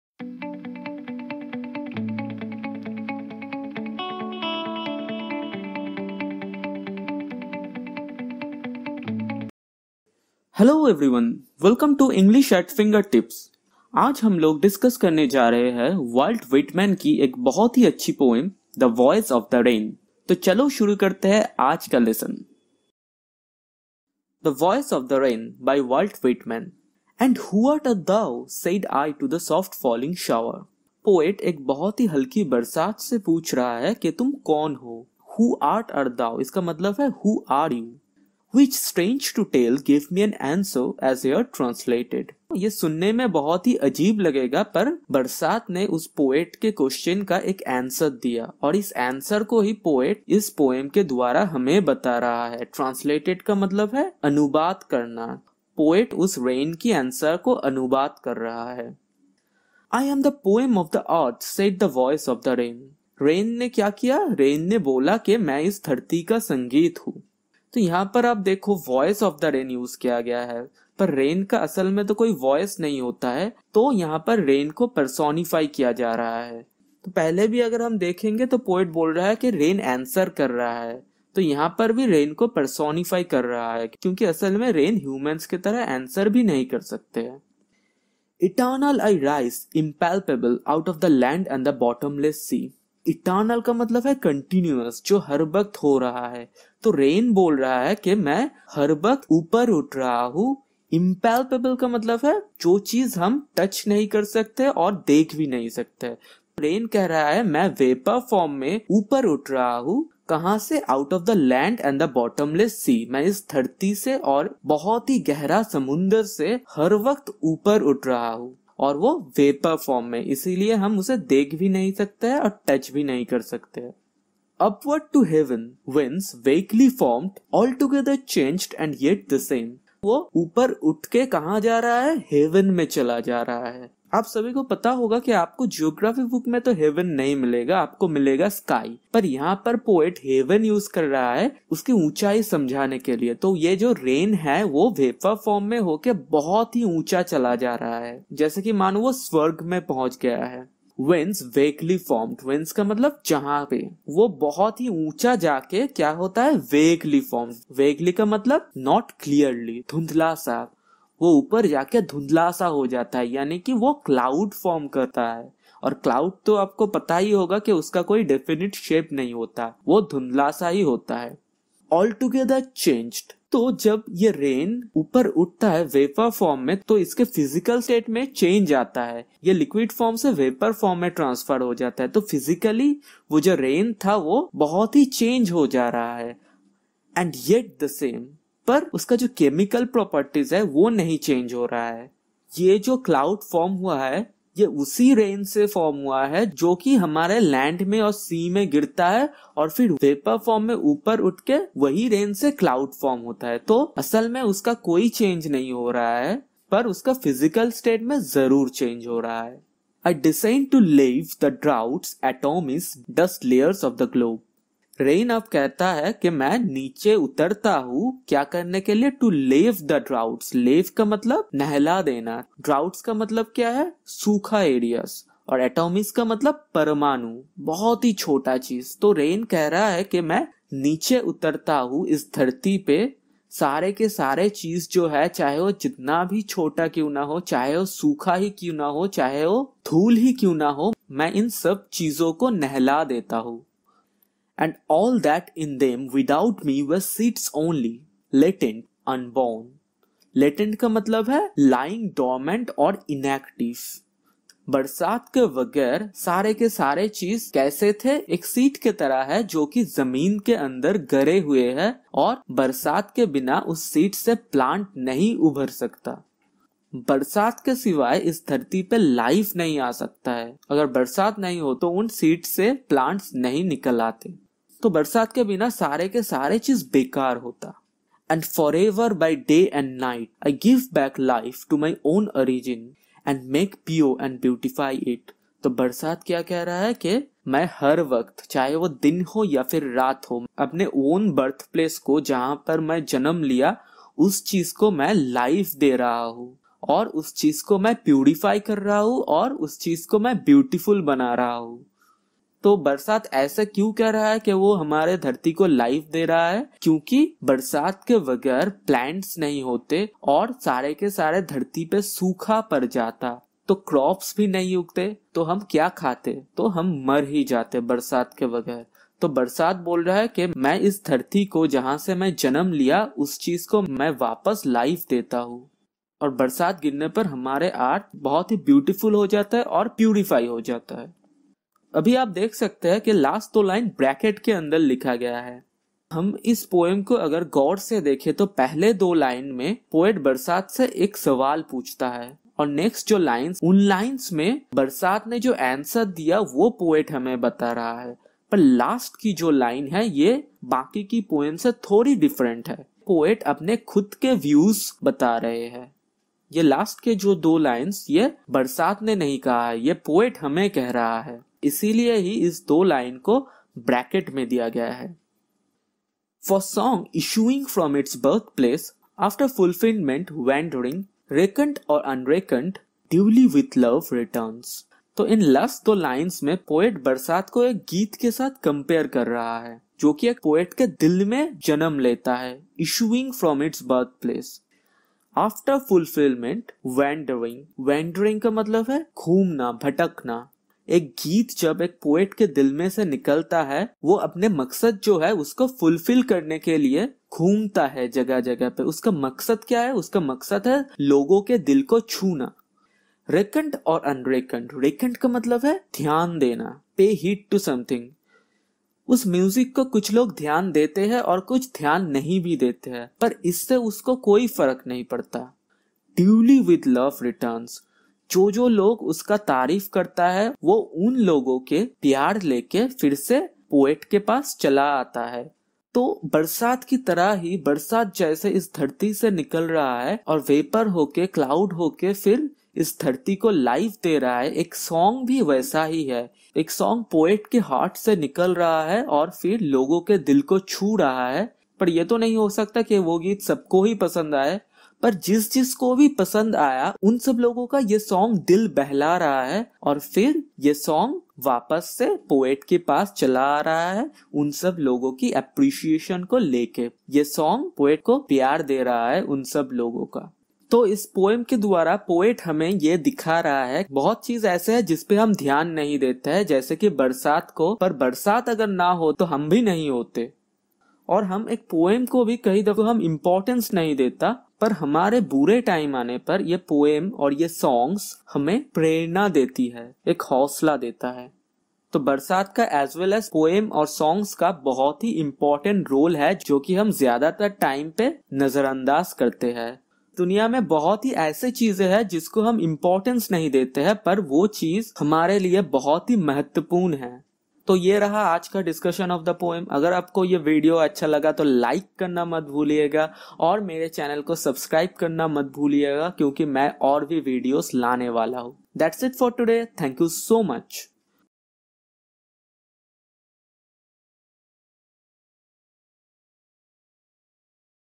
CBSE 11 || The Voice Of The Rain by Walt Whitman line by line HINDI ANALYSIS ||English At Fingertips
This video gives one the line by line explanation as well as analysis in Hindi of Walt Whitman's poem " The Voice of the Rain ", which is very important for CBSE class 11 English Exam.